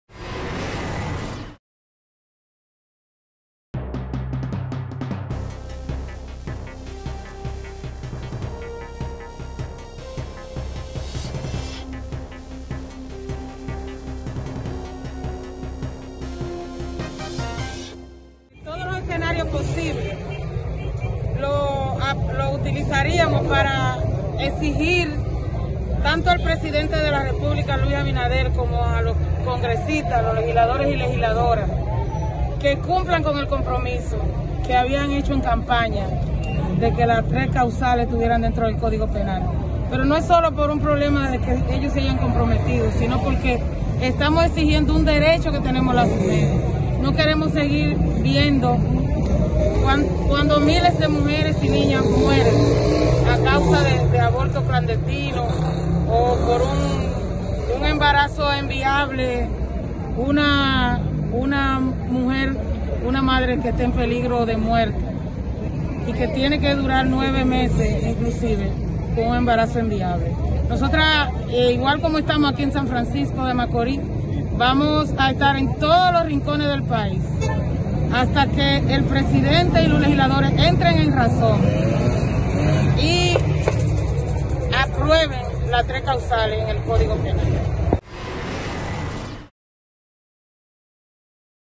Aprovechan visita del presidente Abinader en SFM para pedir las tres casuales en el Código Penal
San Francisco De Macorís.- Un grupo de personas se concentró para exigir las tres causales de la interrupción del embarazo. aprovechando la visita del presidente Luis Abinader.

Este grupo, mayoritariamente compuesto por de mujeres, aprovecharon que el presidente se encuentra en esta ciudad realizando una visita para exigir que las tres causales sean incluidas en el código penal.

La protesta se realizó frente a las instalaciones de la ciudad agropecuaria, donde el presidente tenía previsto la realización de un consejo de gobierno.

Abinader realiza Durante este domingo 25 de abril una visita a este municipio y a otras localidades de la provincia Duarte.